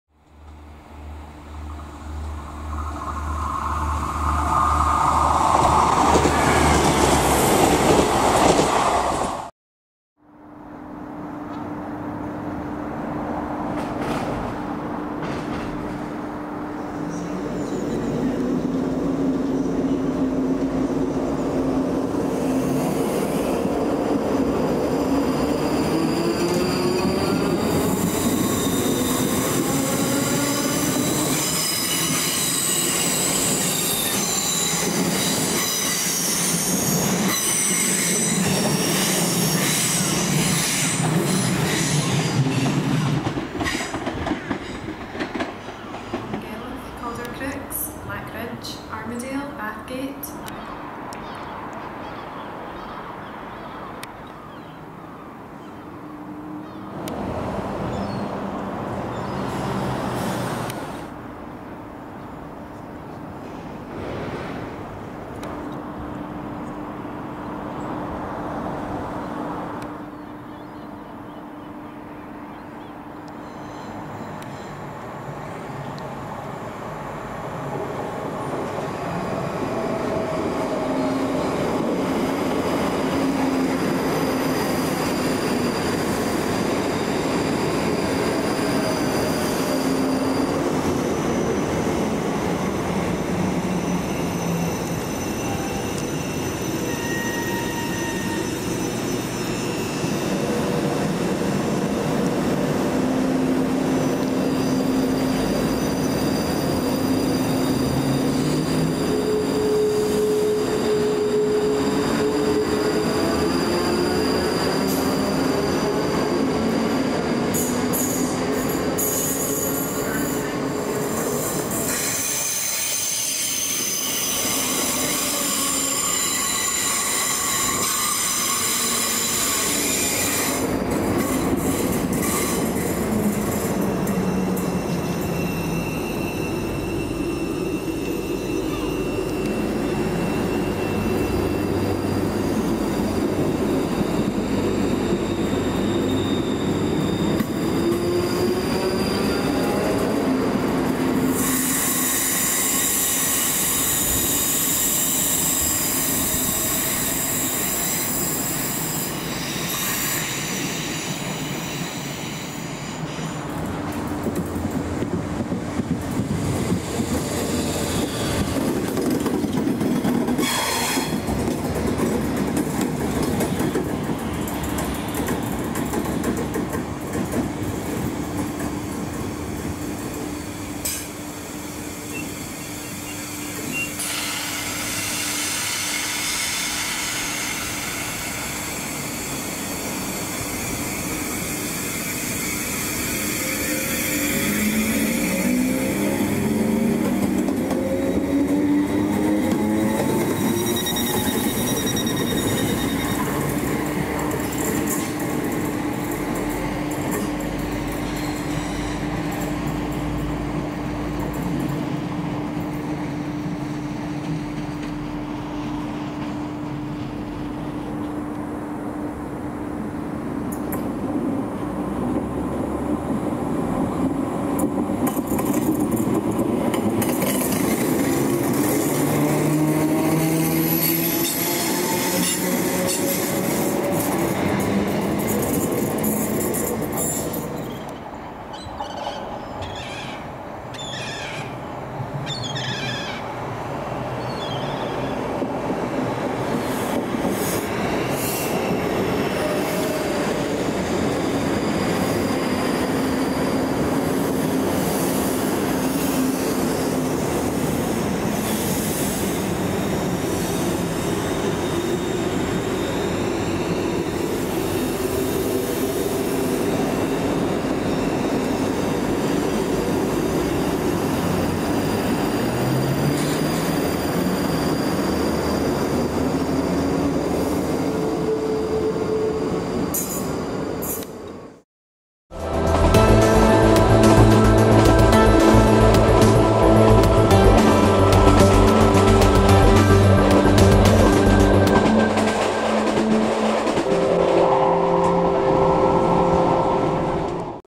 Coatbridge Sunnyside Station 5/8/22
Coatbridge Sunnyside Station is located on Scotrail's Glasgow Suburban Rail network; with only Scotrail services calling here

The station itself is located on the line between Glasgow Queen Street and Airdrie and Edinburgh

Services at the time of filming were all operated using Class 318/320 and Class 334 Electric Multiple Units:

The Class 318 and Class 320's are currently on the cards to be withdrawn from service soon, with a new contract being up for tender to build brand new trains for the Glasgow Suburban Network whilst this was being filmed: